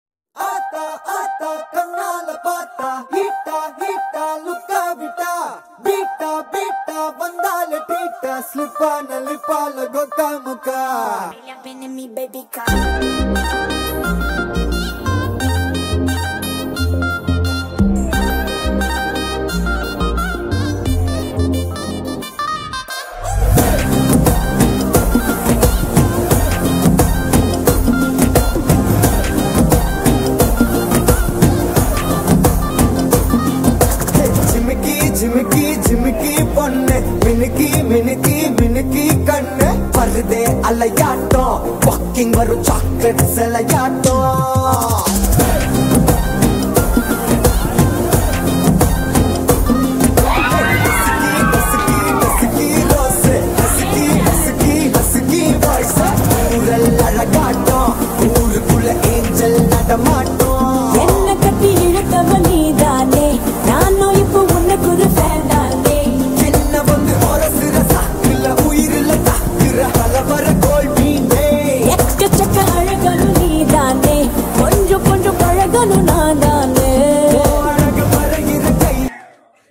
Thumbs Up Drop Challenge 😂
Thumbs Up drop Challenge with all friends. This is fun challenge with friend. Every person pour thumbs up in glass by one by one. When thumbs up came out  game completed. Last person will have some banishment.